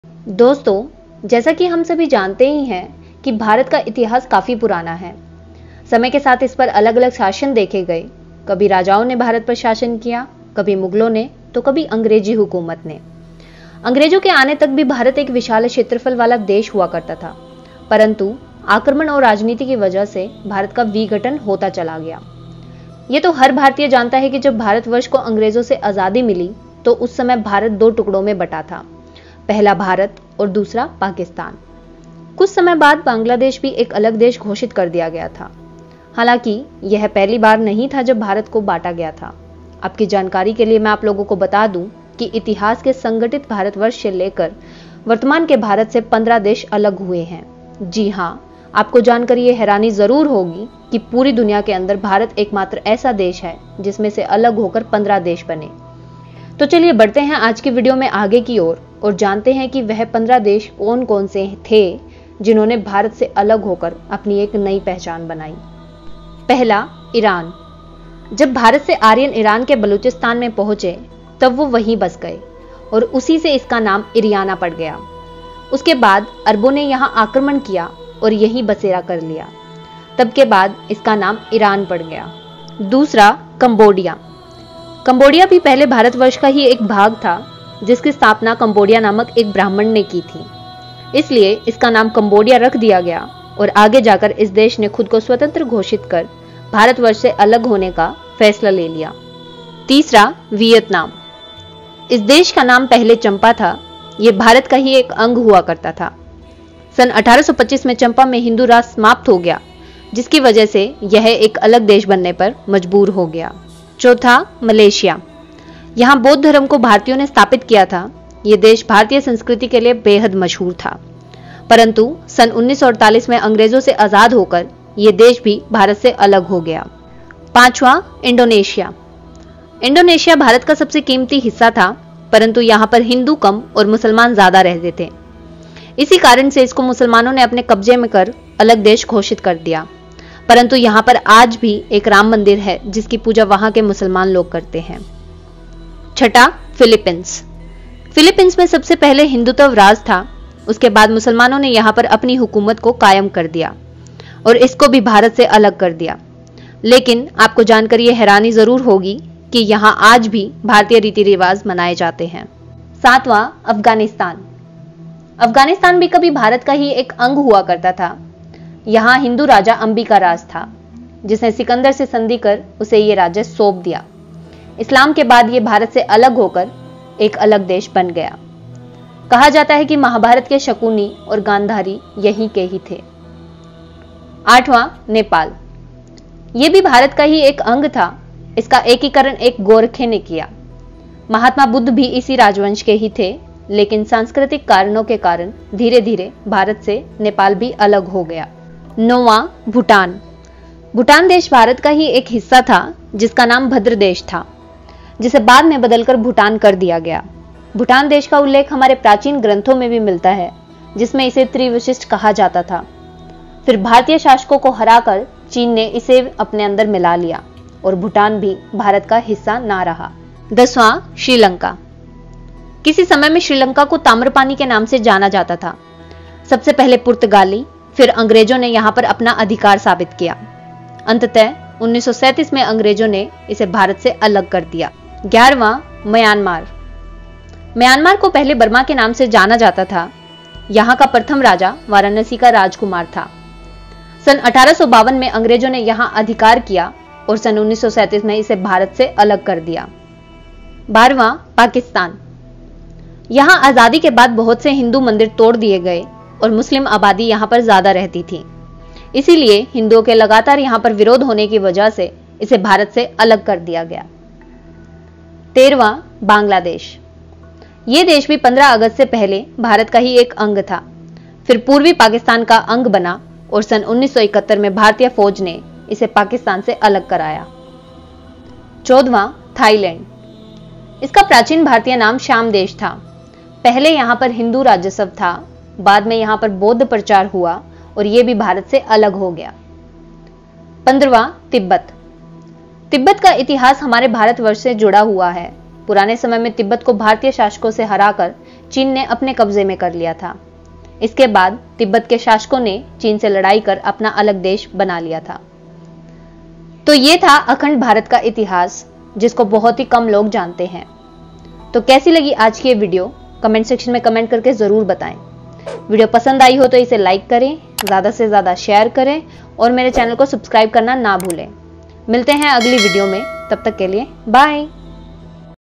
भारत से टूटकर बने हैं ये 15 अलग देश
ये हैं वो 15 देश जो भारत का ही हिस्सा थे!

See you in the next different and interesting Vlogs.❤️

Upasana kadyan

New uploads on every MONDAY🌸